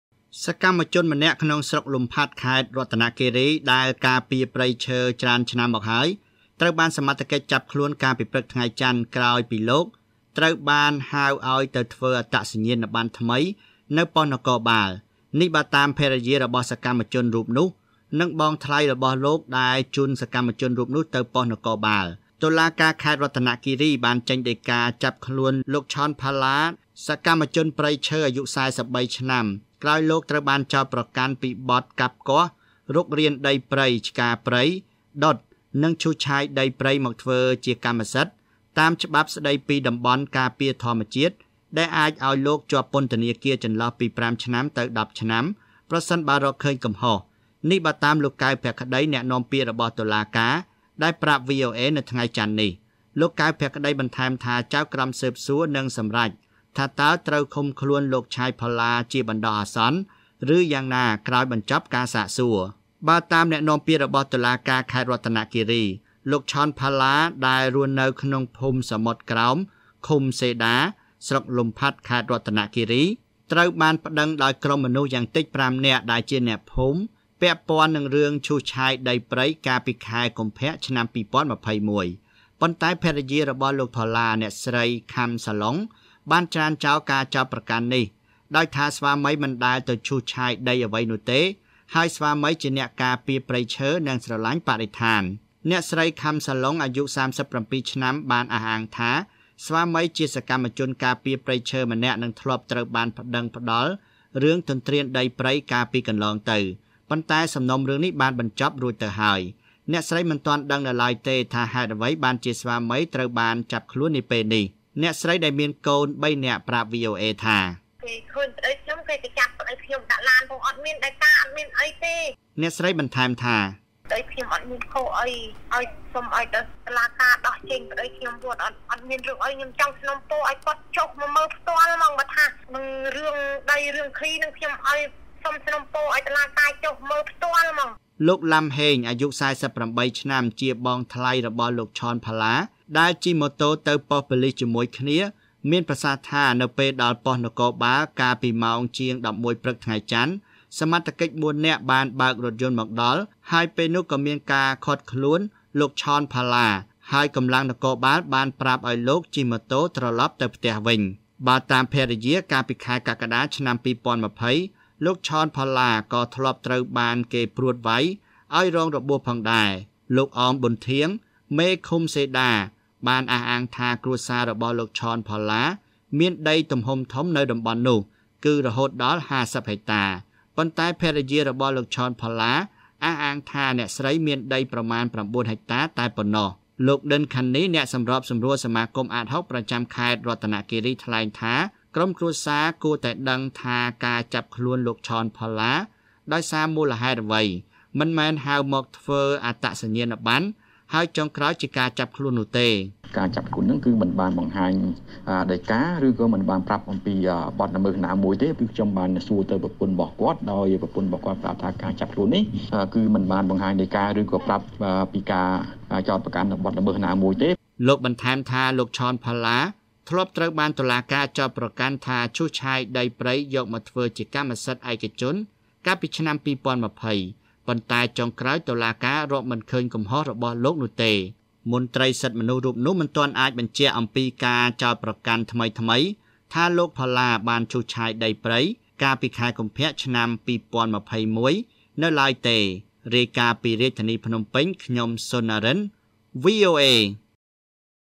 តុលាការឲ្យចាប់ខ្លួនសកម្មជនព្រៃឈើម្នាក់នៅខេត្តរតនគិរី រងការចោទប្រកាន់ថាទន្រ្ទានដីព្រៃ
សកម្មជនព្រៃឈើម្នាក់ក្នុងខេត្តរតនគិរីលោក ឆន ផល្លា ត្រូវបានឃាត់ខ្លួន កាលពីព្រឹកថ្ងៃចន្ទ ដោយក្រុមគ្រួសារមិនទាន់ដឹងថាលោកត្រូវបានចោទប្រកាន់ពីបទអ្វីនោះទេ។ ភរិយារបស់សកម្មជនរូបនោះអះអាងថាការចាប់ខ្លួននោះមិនមានដីកាបង្ហាញទៅក្រុមគ្រួសារឡើយ ដោយប្រើប្រាស់វិធីសាស្រ្តហៅស្វាមីទៅធ្វើអត្តសញ្ញាណបណ្ណនៅប៉ុស្តិ៍នគរបាល ប៉ុន្តែលោកត្រូវបានឃាត់ខ្លួននៅទីនោះ។ ប៉ុន្តែមន្រ្តីតុលាការឲ្យដឹងថាលោកត្រូវបាននាំមកសាកសួរអំពីការកាប់គាស់រុករាន និងឈូសឆាយដីព្រៃមកធ្វើជាកម្មសិទ្ធិ។

ទស្សនា​វីដេអូ​របស់​យើង​ខ្ញុំ​តាម​ប្រធានបទ​ខាង​ក្រោម៖